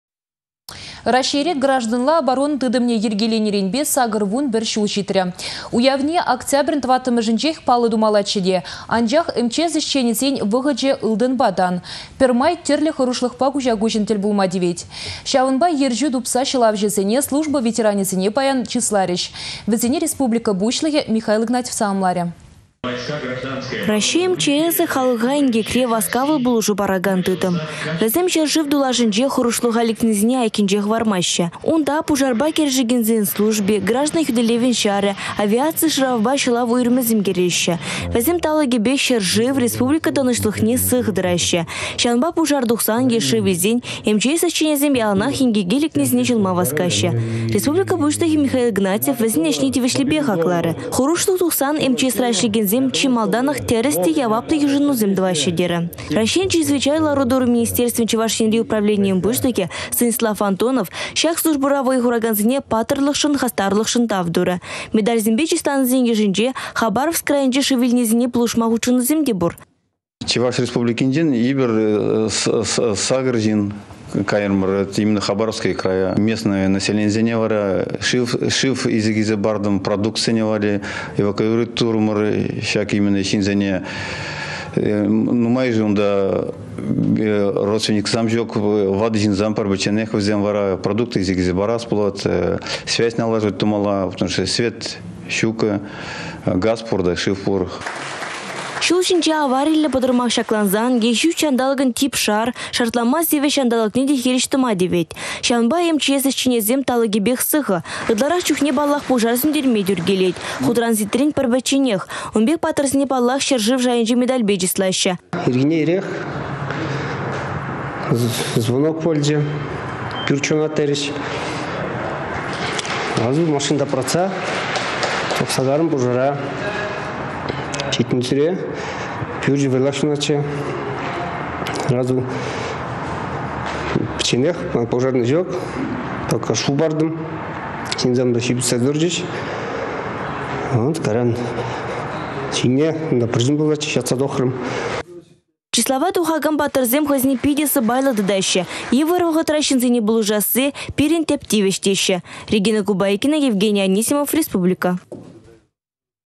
Раççей МЧСĕн Граждан хÿтлĕхĕн кунĕ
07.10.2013
Раççейре Гражданла оборона тытăмне йĕркеленĕренпе  81 çул çитрĕ. Уявне октябрĕн 4-мĕшĕнчех паллă тумаллаччĕ те, анчах МЧС ĕçченĕсен вăхăчĕ ылтăнпа тан - пĕрмай тĕрлĕ хăрушлăхпа куçа куçăн тĕл пулма тивет. Çавăнпа ерçÿ тупса çăлавçăсене, служба ветеранĕсене паян чысларĕç. Вĕсене Республика пуçлăхĕ Михаил Игнатьев та саламларĕ.